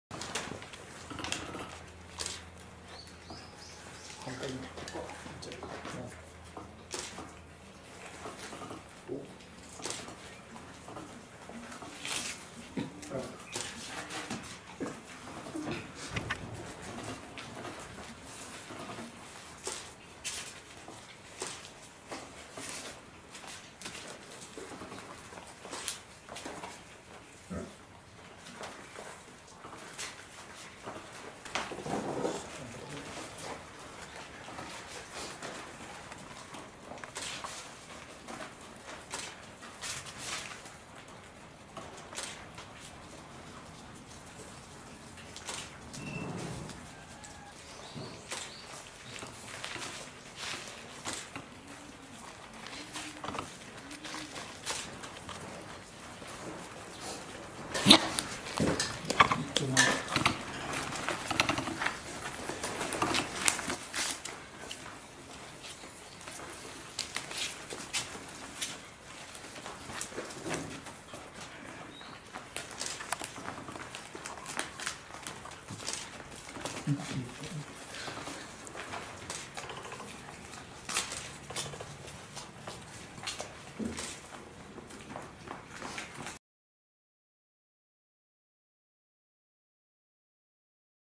てるくん☆がんばりました
「南阿蘇ふれあい動物園フェアリーテール」
カピバラのスミちゃん。今日のおやつ、旬なトウモロコシをいただきま～す！
するとすぐにてるくんが参戦！
いつもスミちゃんに弱腰なてるくんですが、よほど美味しかったのかこの日はがんばってましたが、ワイルドなスミちゃんもトウモロコシ振り回して応戦！
そして気が付くのが遅すぎるアーリーちゃんも参戦してみんなで仲良く食べました～
こうやって、厳しさも覚えながら成長していくのですね～